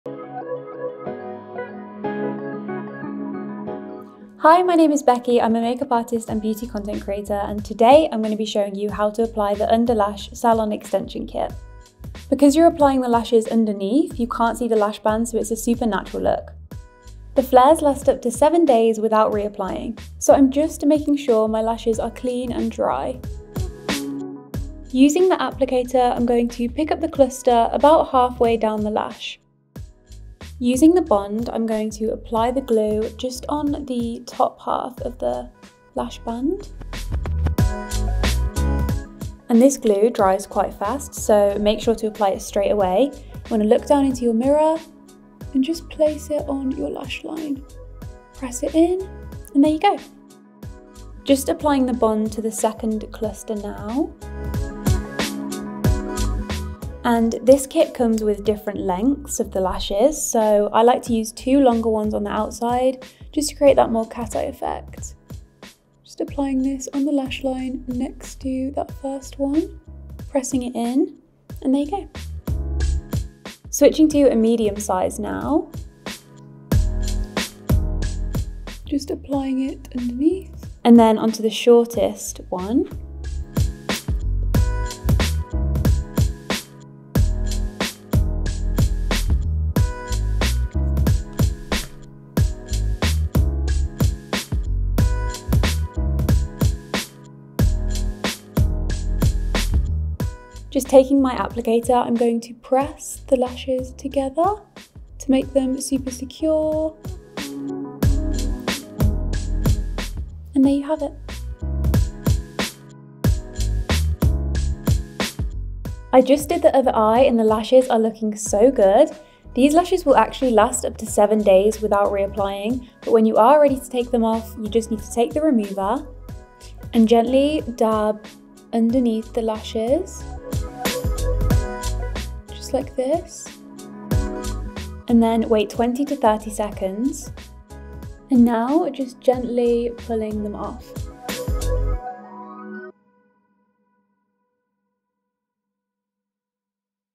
How To Apply Eylure's UNDERLASH Salon Extension Kit!
Eylure Underlash Salon extension kit is an innovative new way to get a salon finish lash look at home. This extension kit allows you to adhere lash flares under your natural lashes for an Incredibly natural long-lasting lash look.

This 1 step kit includes 30 wispy lash flares, a bond, an applicator, and remover so you have everything you need to create a stunning salon lash extension look in the comfort of your own home.

The lash flares are easy to apply and feel extremely light weight and comfortable on the eye. You will get up to 5 applications per kit and once applied the lash flares will last up to 7 days.